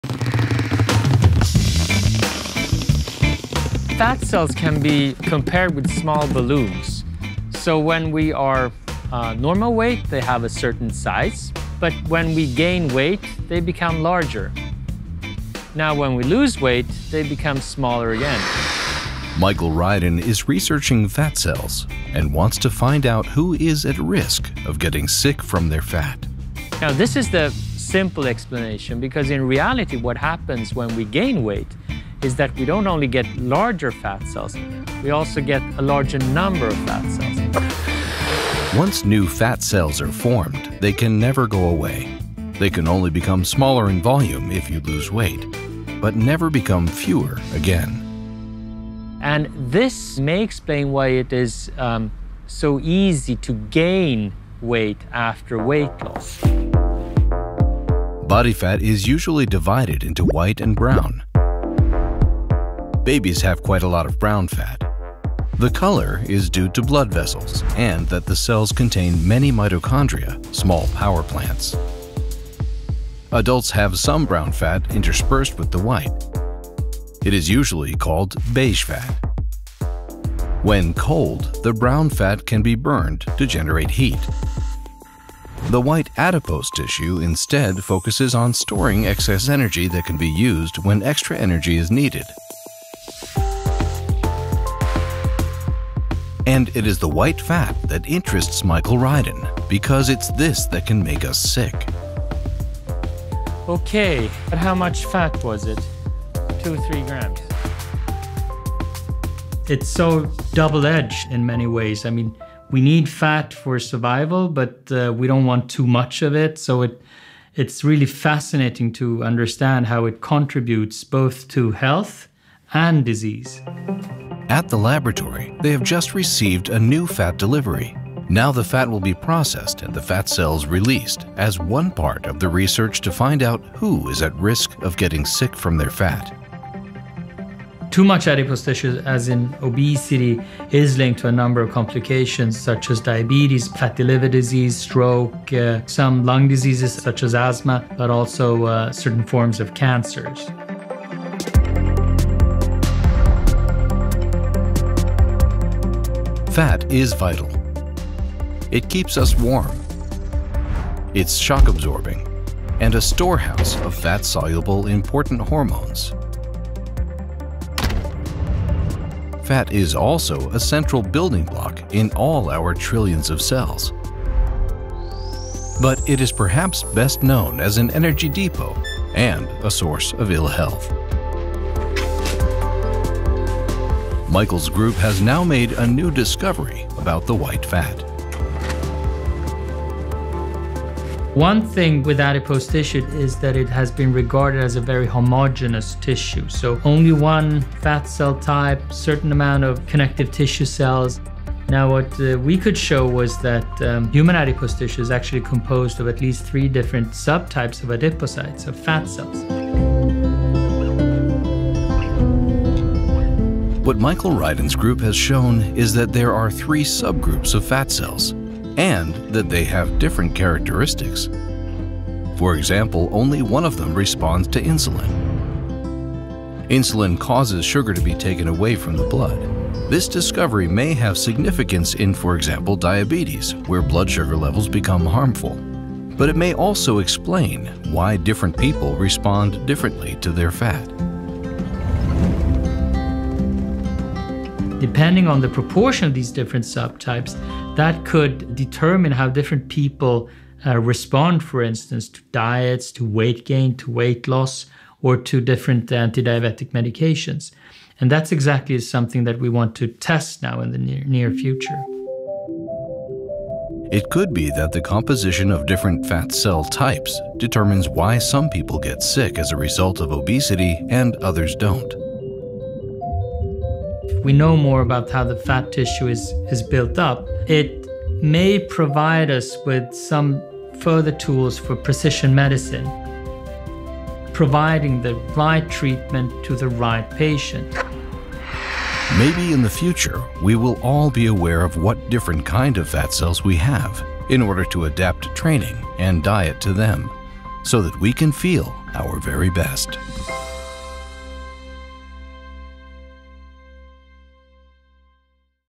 Who is getting sick from their fat cells?
Mikael Rydén, Karolinska Institutet, is researching fat cells and wants to find out who is at risk of getting sick from their fat. Diabetes, fatty liver disease, stroke, some lung diseases, but also certain forms of cancers are some examples. Rydén's group has shown that the white fat is much more varied than previously thought, that there are three subgroups of fat cells, and they have different characteristics. This may provide to further tools for precision medicine. Providing the right treatment to the right patient. Maybe will we also be able to adapt training and diet to the different cell types.